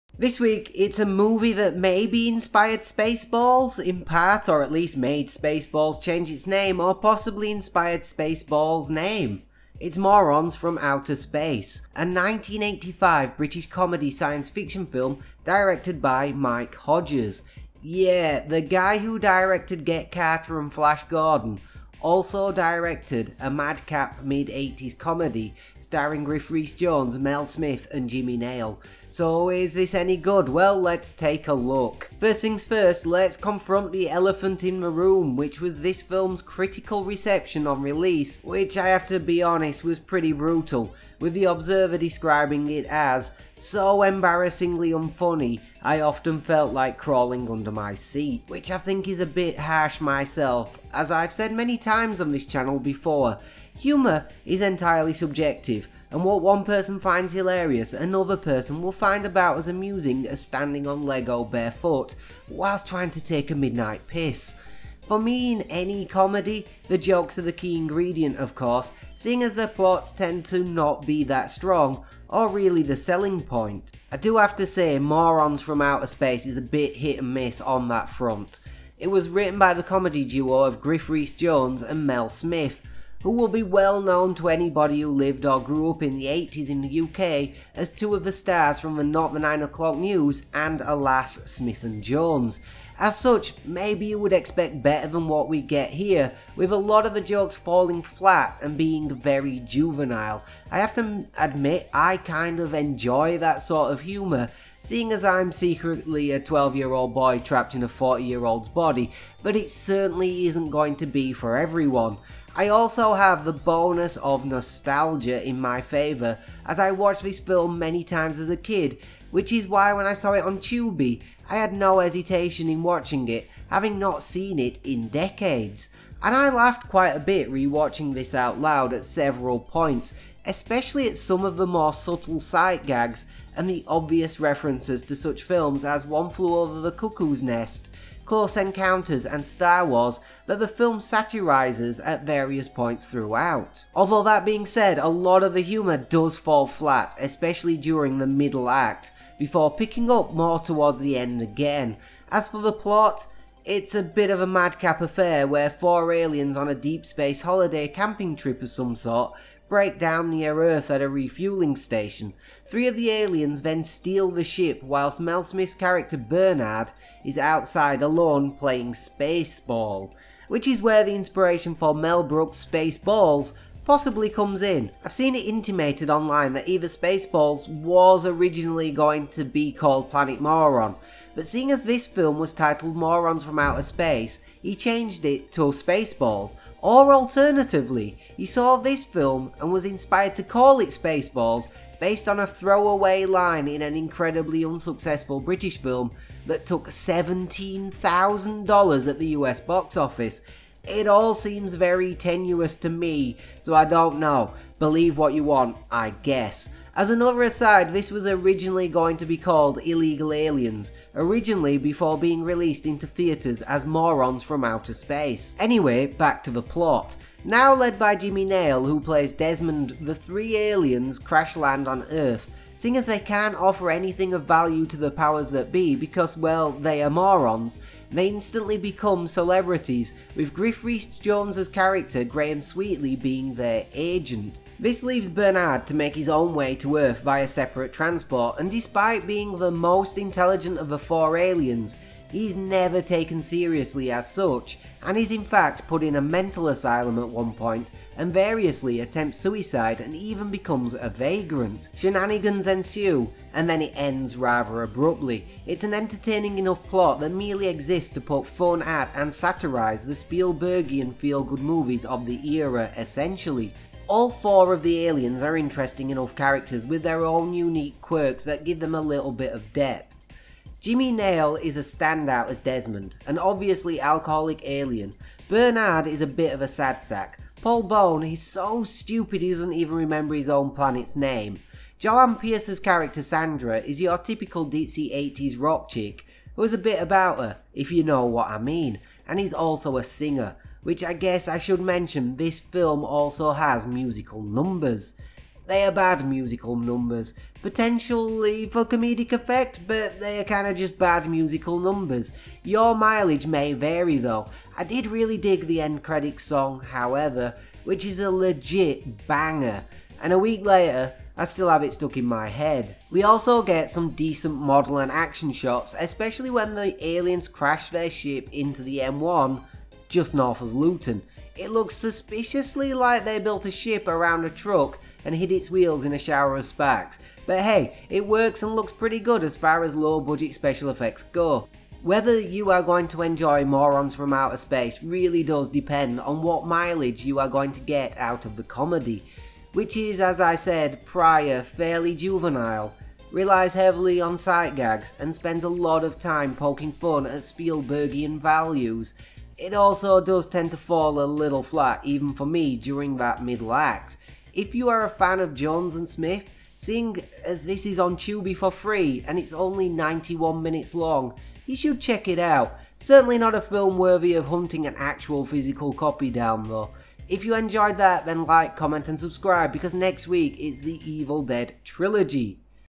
Morons from Outer Space a film that maybe inspired Spaceballs a Retrospective review
A retrospective review of Morons from outer Space, a 1985 British comedy science-fiction film directed by Mike Hodges.

Yeah the guy who directed Get Carter and Flash Gordon also directed a madcap mid-80’s comedy starring Griff Rhys Jones, Mel Smith and Jimmy Nail.

A movie that maybe inspired Spaceballs in part or at least made Spaceballs change its name or possibly inspired Spaceballs name…

So is this any good and how exactly if at all did it inspire and influence Spaceballs, well let’s take a look?

0:00 - Intro
0:32 - The Elephant in the Room
0:48 - The Humour
2:32 - The Plot
2:50 - The Spaceballs inspiration or is it?
3:35 - The plot continued
4:24 - The Characters
4:51 - Oh it's a musical now
5:12 - The special effects
5:30 - In conclusion
6:07 - Outtro